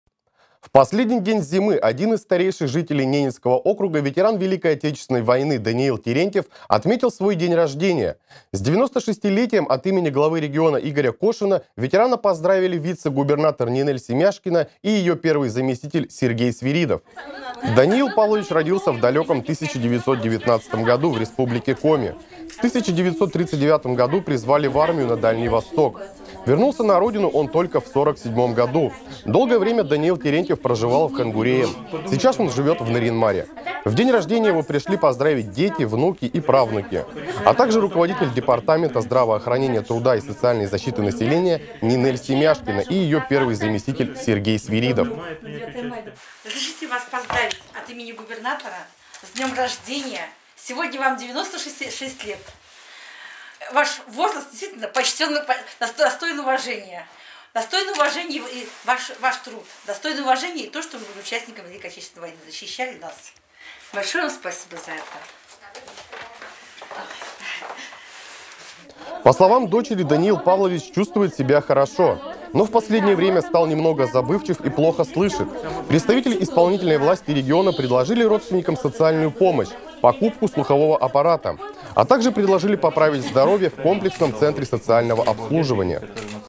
Ветеран Данил Терентьев отметил 96-й день рождения
В последний день зимы один из старейших жителей Ненецкого округа, ветеран Великой Отечественной войны Данил Терентьев отметил 96-й день рождения. С этой датой от имени главы региона Игоря Кошина его поздравили вице-губернатор Нинель Семяшкина и ее первый заместитель Сергей Свиридов.